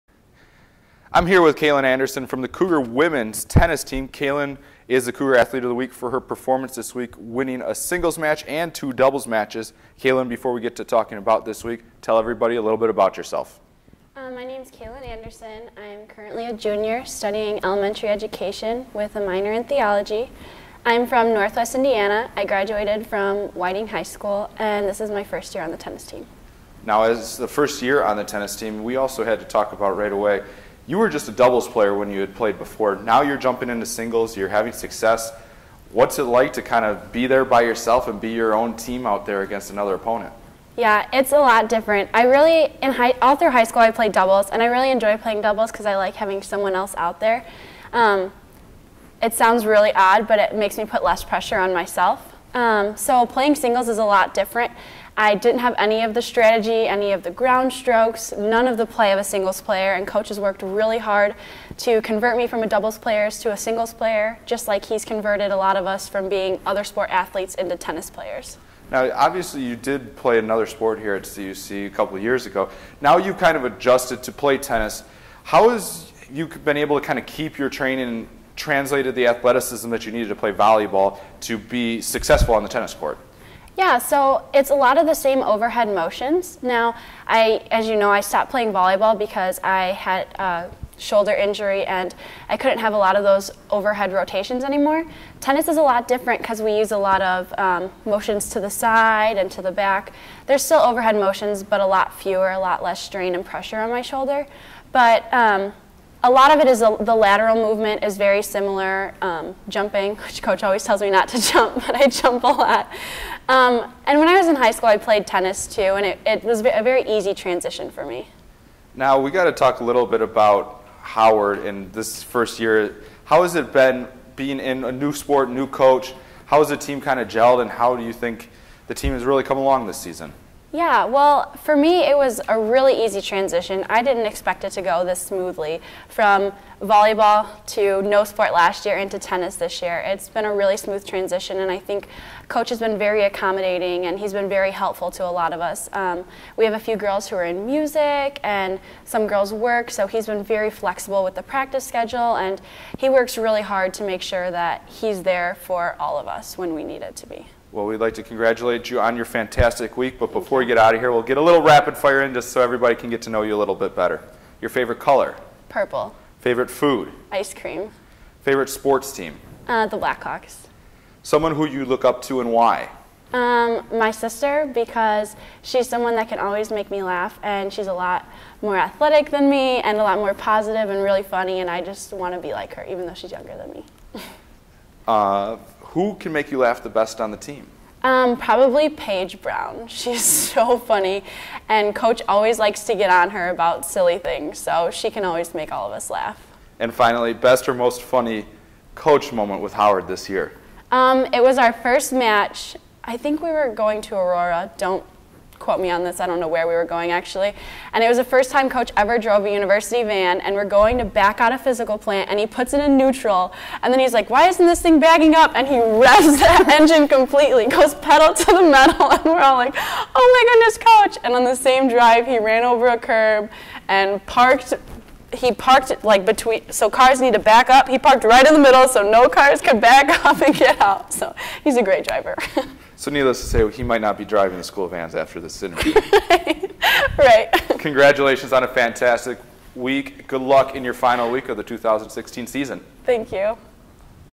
CUC Athlete of the Week Kaylyn Anderson
Kaylyn is the CUC athlete of the week after picking up two doubles wins and a singles match win this week for the Women's Tennis team